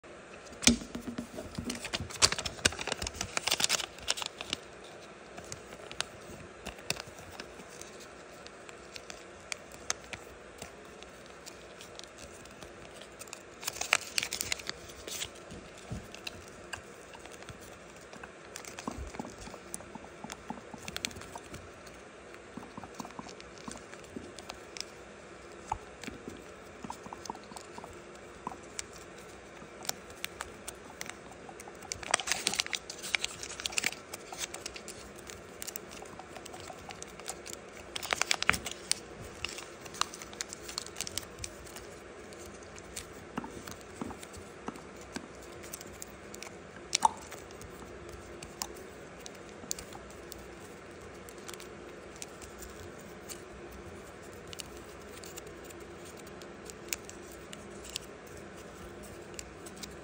Turning Paper into Gold Origami Paper Crane Asmr
Turning paper into Gold by folding Asmr Origami.